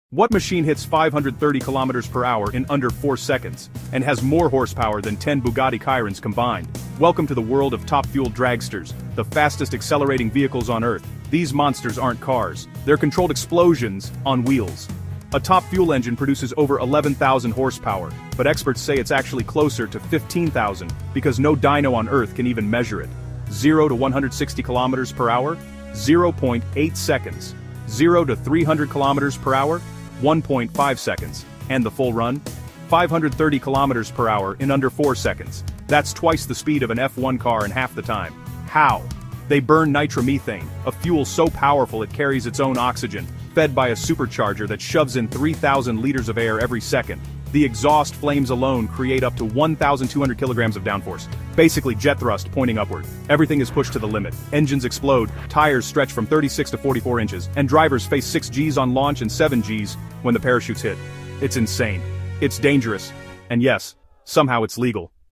This Thing Out-Accelerates a Jet 😳🔥
Top Fuel Dragsters are the fastest-accelerating machines on the planet — hitting 530 km/h in under 4 seconds with over 11,000 horsepower, burning nitromethane fuel, and generating jet-engine levels of thrust. Here’s how these monsters bend physics, reality, and common sense. 🔥